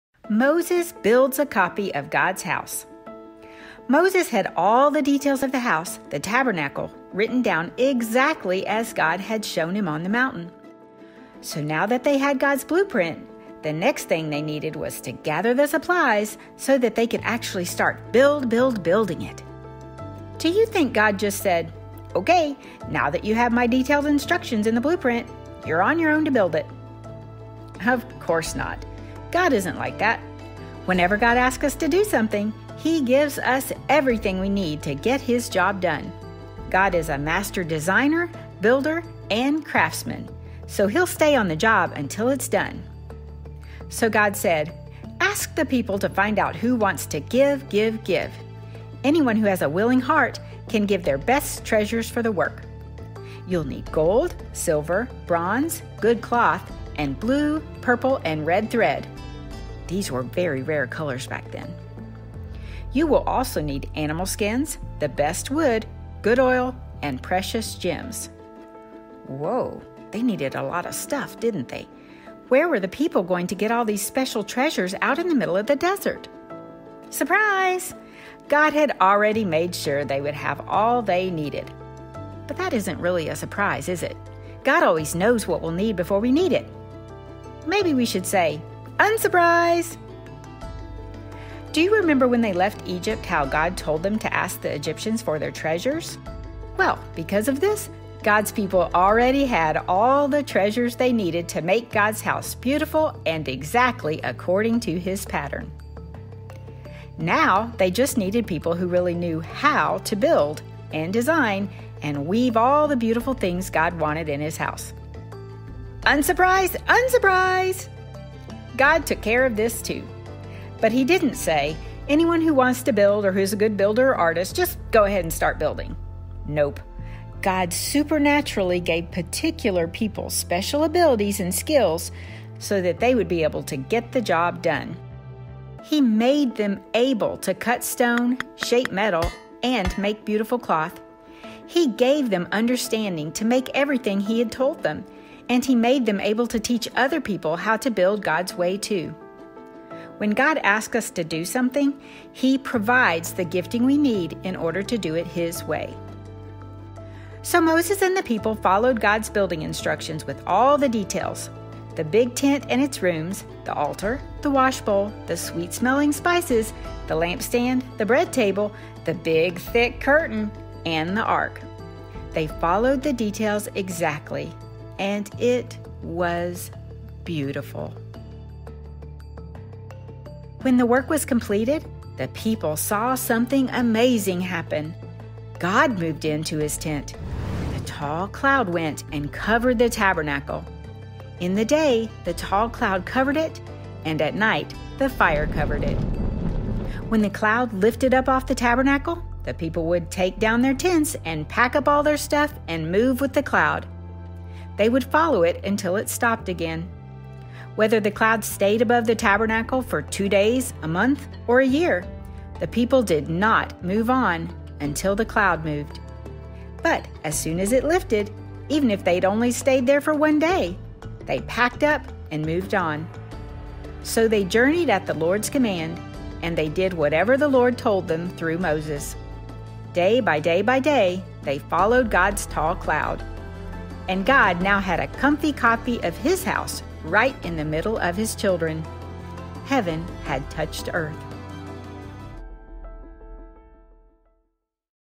Day 48 Moses Builds a Copy of God's House ~ Daily Bible Stories for Children & Learners
When God asks us to do something, He provides the supernatural gifting we need in order to do things His way.